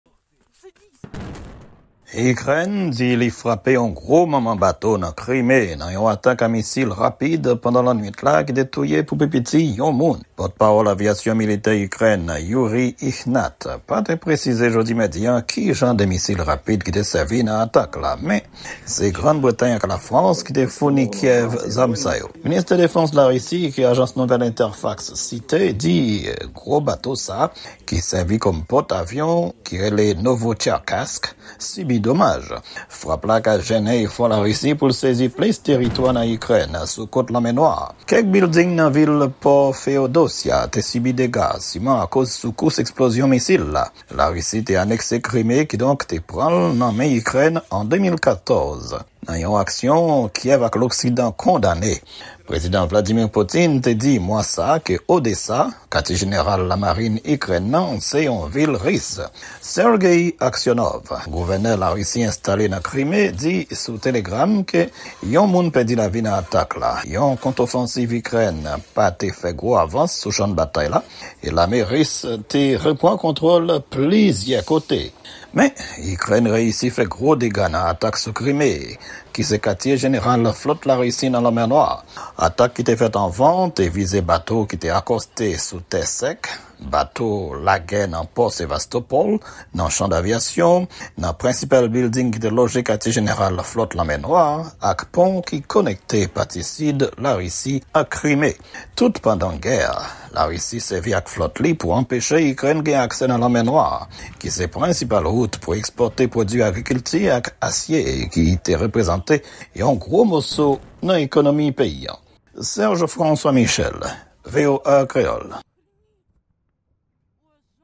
Ikrèn Atake yon Bato de Gè Ris
Selon otorite Ikrenyen yo atak la andomaje bato Ris la ki te estasyone nan Prèskil Krime a. Otorite yo nan Kremlen rekonèt bato a sibi domaj. Pèson pa konnen ki tip za m Ikrèn itilize nan atak la